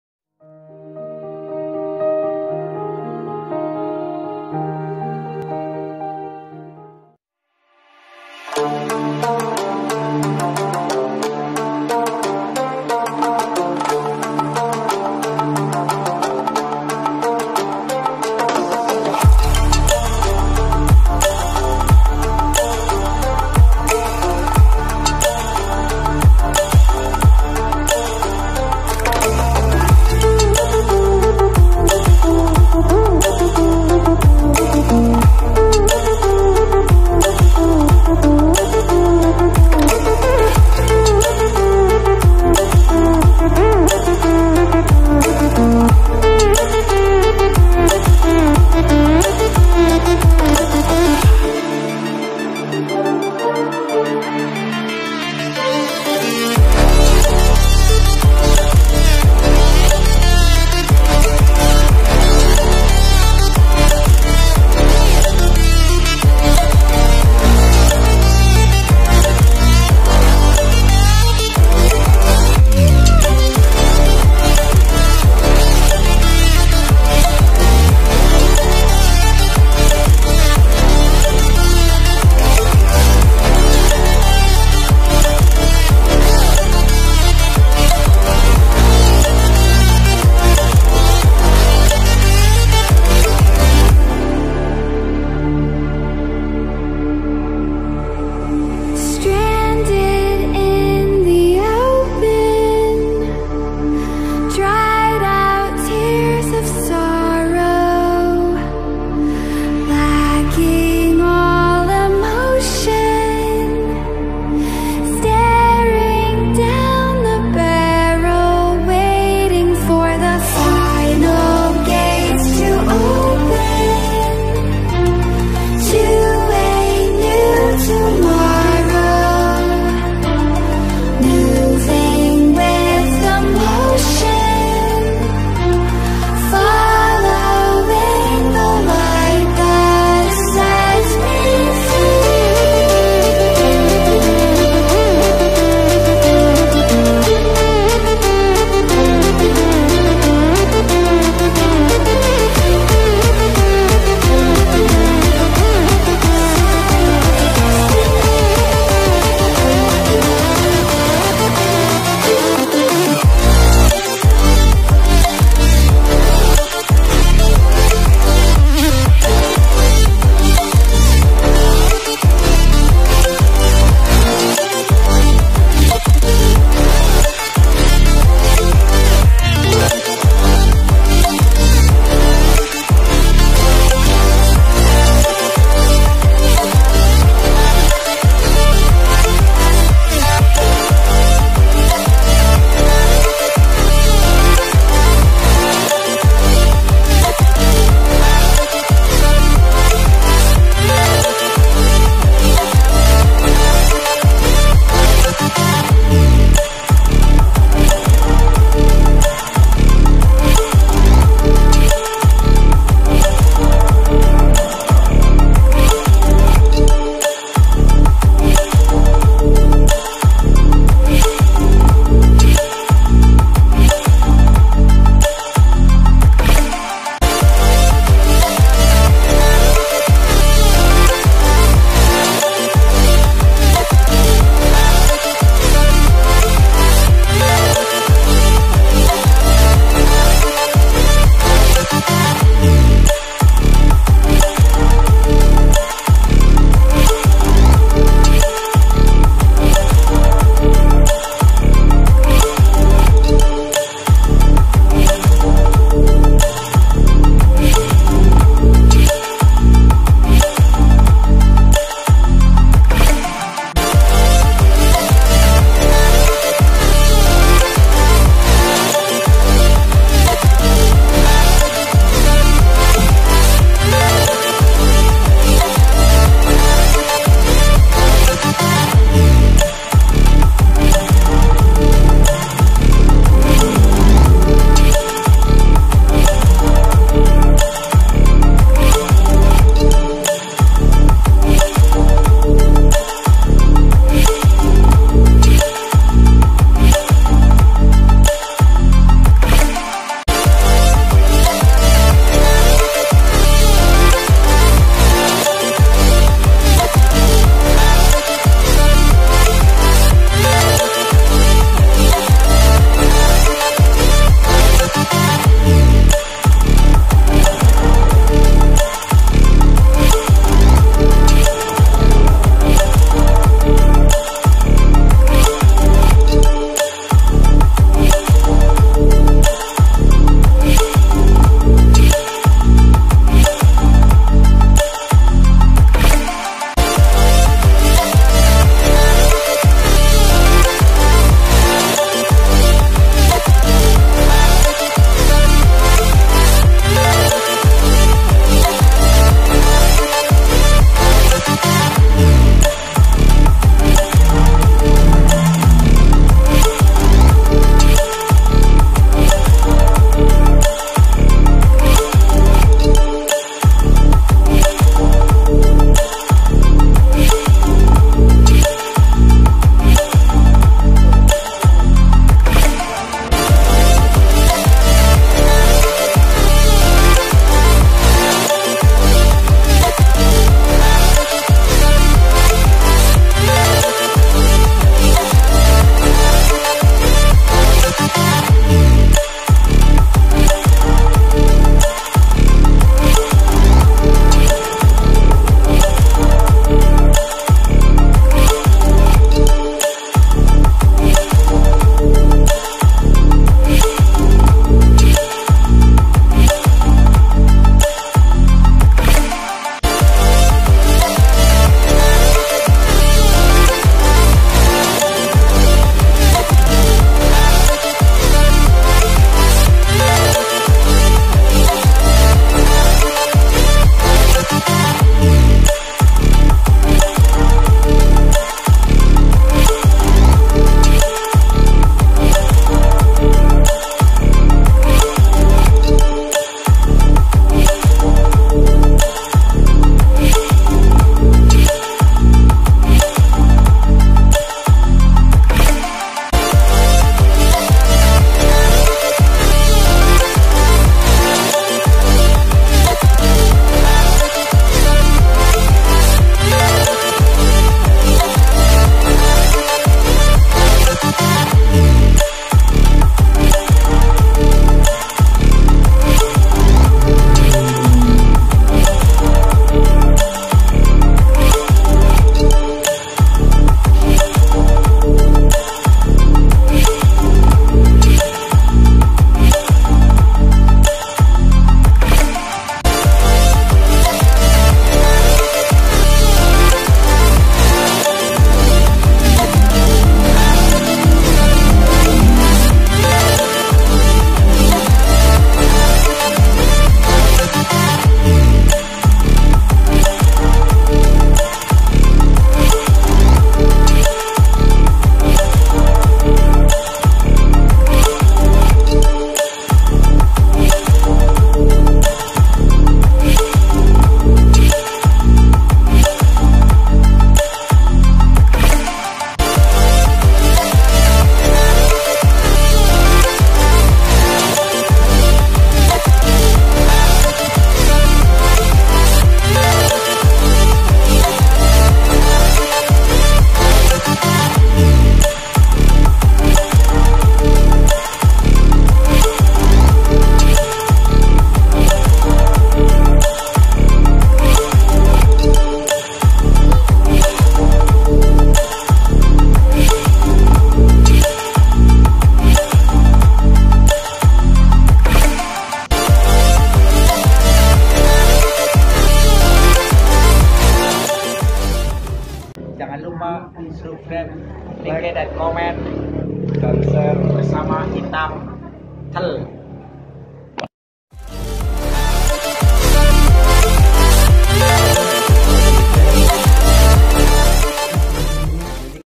Cat BUNGLON cara mengoplosnya | Mixing Paint Car
Assalamualaikum teman teman semuanya...Kali ini saya akan berexperimen oplos cat warna Bunglon...oke simak terus videonya ...Thanks